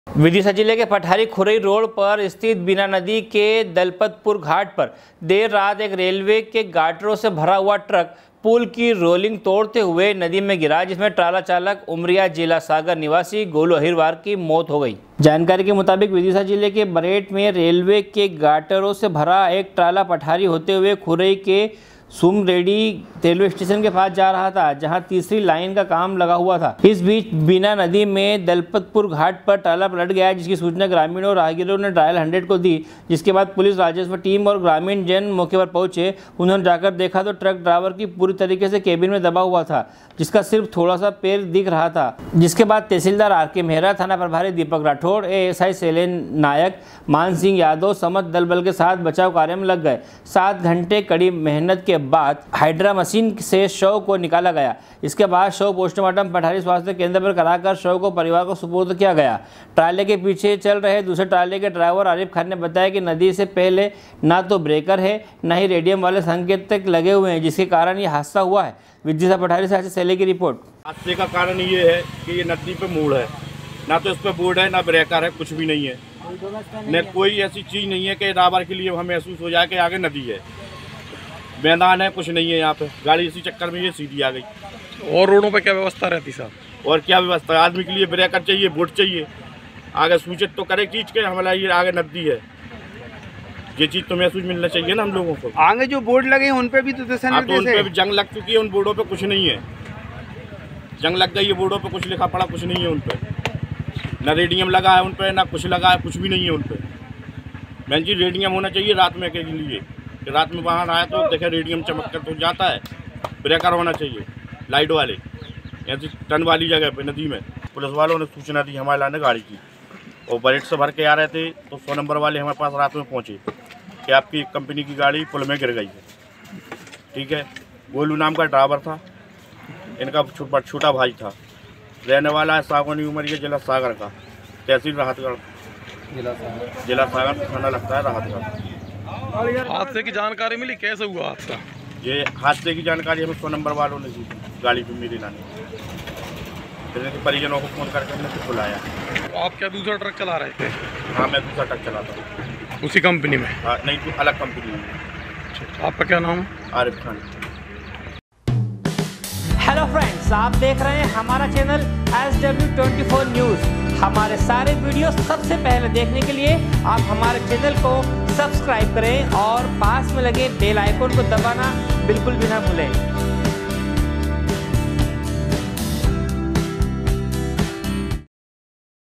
SW 24 News Madhyapradesh
विदिशा जिले के पठारी खुरई रोड पर स्थित बीना नदी के दलपतपुर घाट पर देर रात एक रेलवे के गाटरो से भरा हुआ ट्राला पुल की रेलिंग तोड़ते हुए नदी में गिरा जिसमें ट्राला चालक उमारिया जिला सागर निवासी गोलू अहिरवार की मौत हो गई। विदिशा पठारी से आशीष सहेले की रिपोर्ट